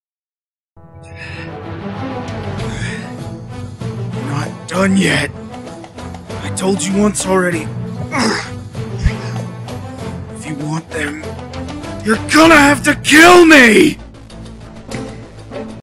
Dalton Dub Self Anime
Yet another short dub.
A character I created based on myself.
Just a short dub test for my own character. Maybe ill give animating it a shot later on and re-post it animated.
All Characters art and Audio Belongs to S8 Studios.
Please ask for permission before using.

Dalton Dubbed by: Takashi Jin Riggs